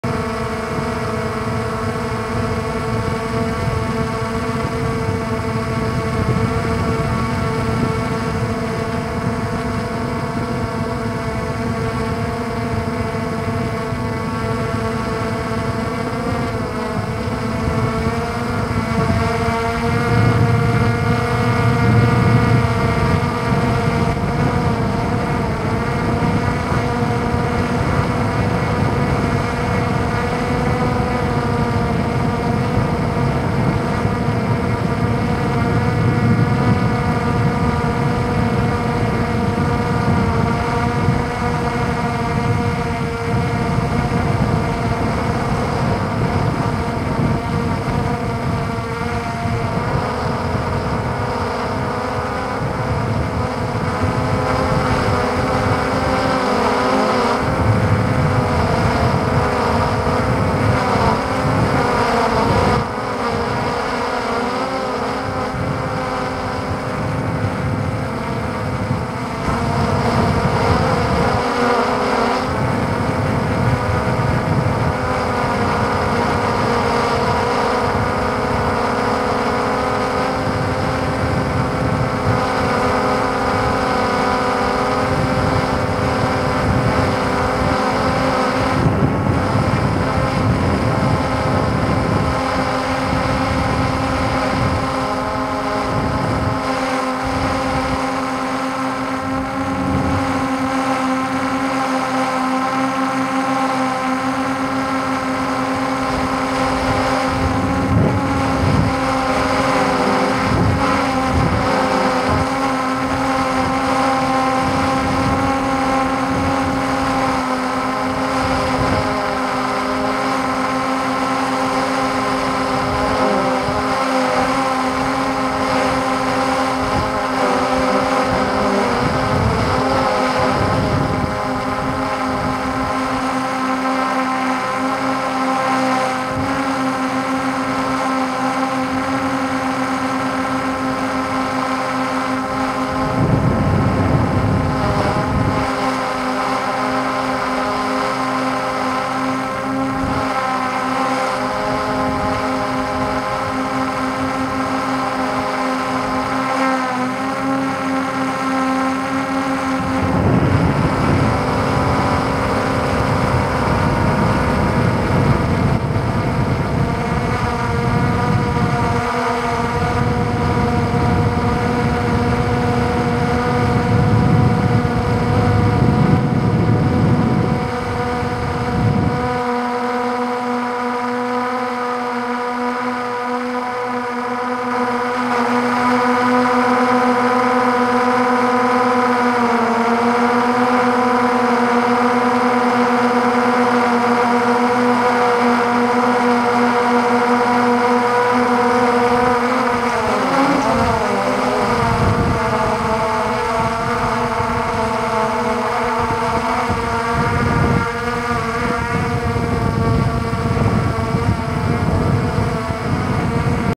QooCam 8K Prores HQ
Prueba de estabilización y calidad.
Grabado en 8K con 10 bit y exportado en ProRes, archivo final 75 GB

Sujetada con el DJI Mavic 2 Pro

Para comprar
🔥🔥🔥 QooCam 8K

🔥🔥🔥 DJI Mavic 2 Pro
⭐️⭐️⭐️ Adaptador para cámara 360